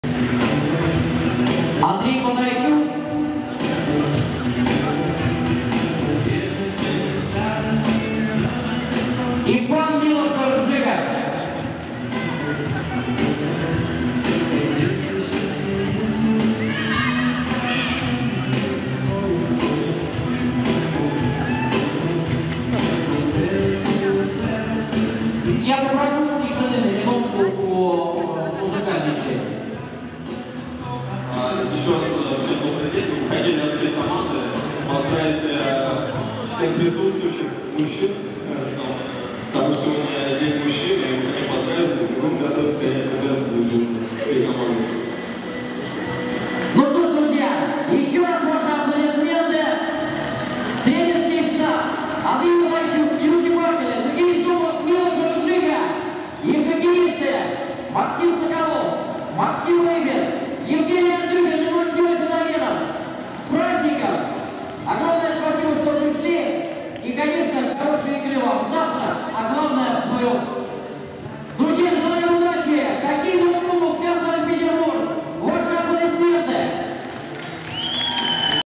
День хоккея в Стокманне
23 февраля в ТК "Невский Центр" состоялась автограф-сессия с хоккеистами и тренерским штабом хоккейного клуба СКА.